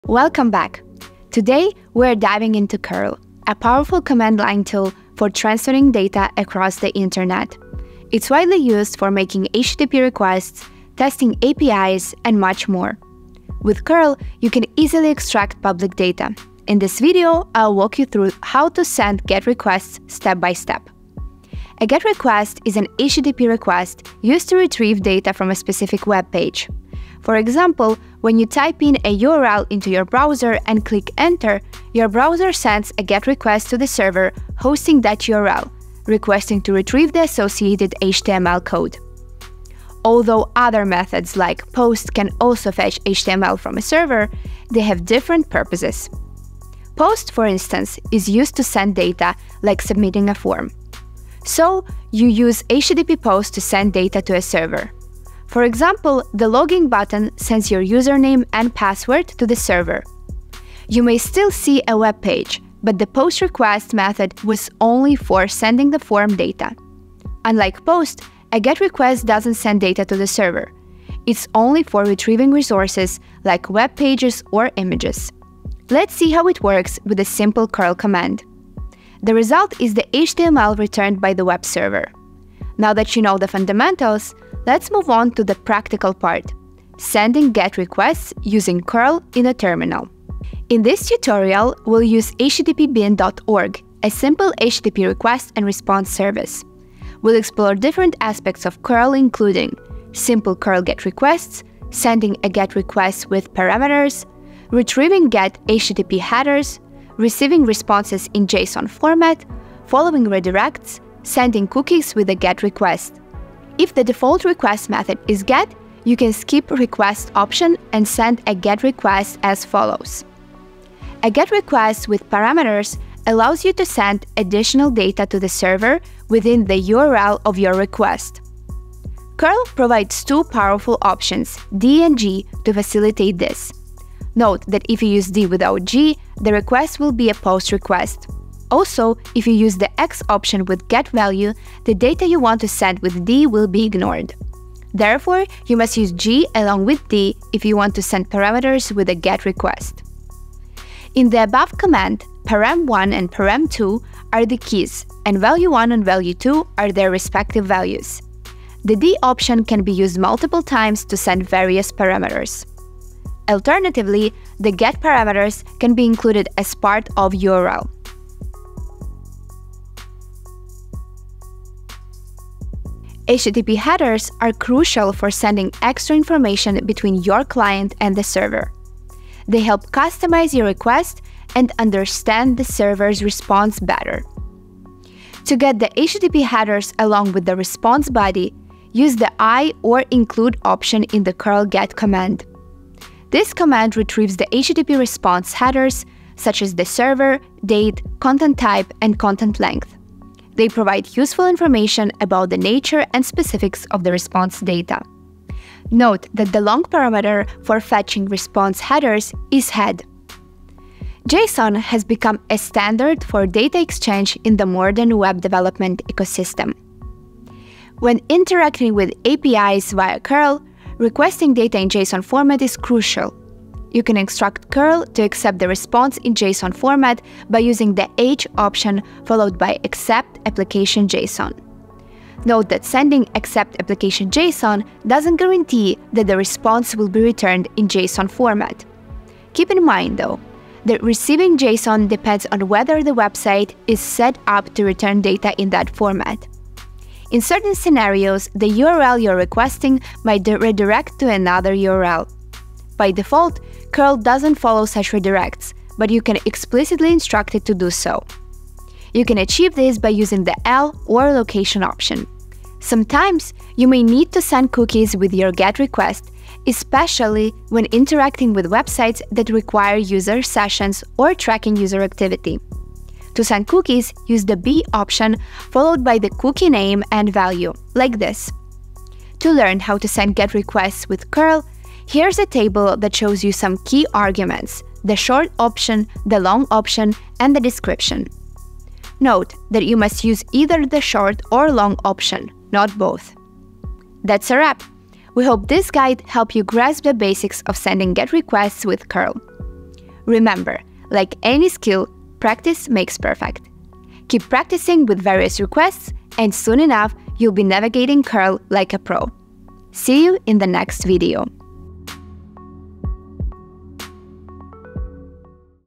How to Send GET Requests With cURL
Ever wondered how to send GET requests with cURL? Watch this video and learn!

In this video, you’ll learn what a GET request is, how it differs from POST request, and how to implement it. Then, you will see the a practical demonstration as well as learn about the key arguments of a GET request.

🔧 OUR SCRAPING SOLUTIONS
Residential Proxies:

⏳ TIMESTAMPS

0:00 Intro
0:20 What is a GET request?
1:25 Steps of sending GET requests with cURL
4:40 Key GET requests arguments
4:50 Outro